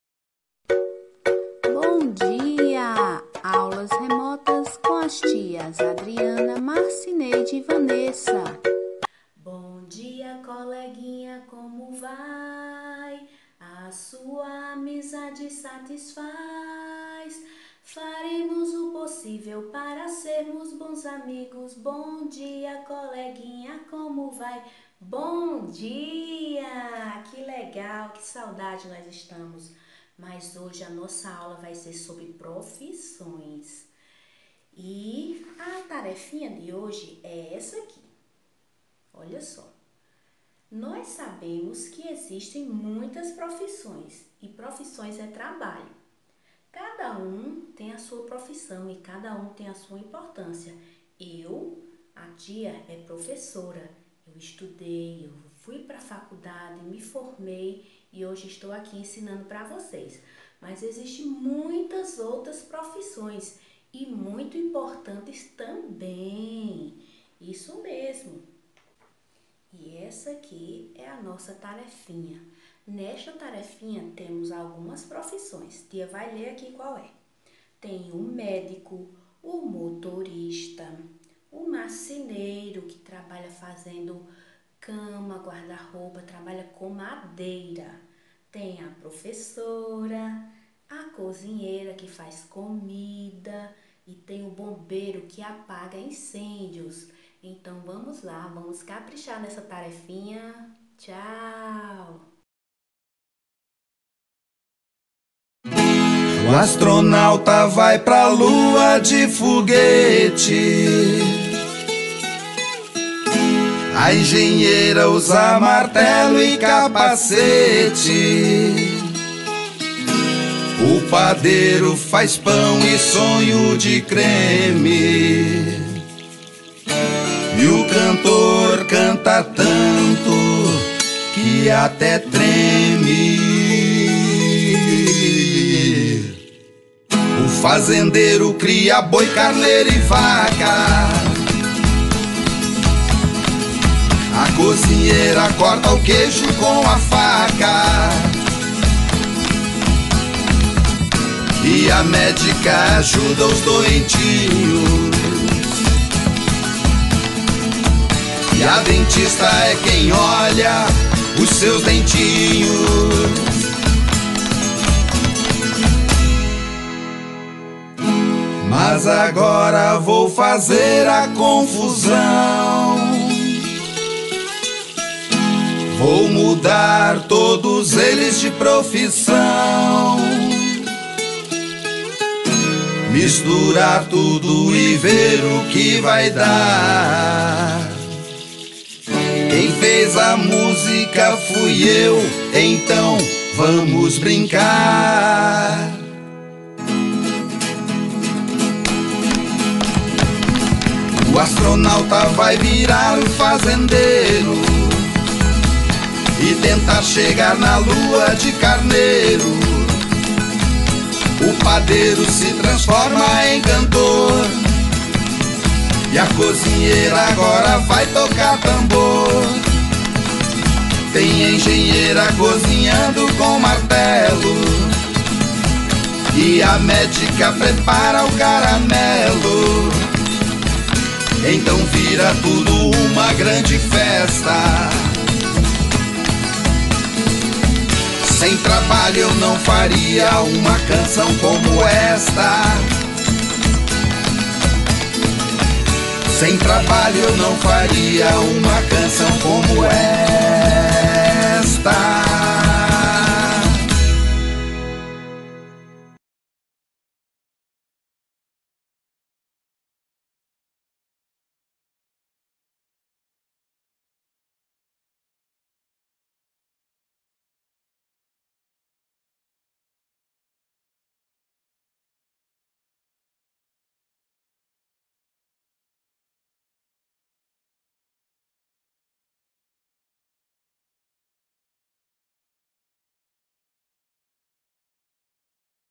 Aula 86 - Dia 10 de Dezembro de 2020. Creche Cremilde Bezerra Wanderley
Aulas Remotas: As profissões
Vídeo utilizado:
Professoras: Adriana, Marcineide e Vanesa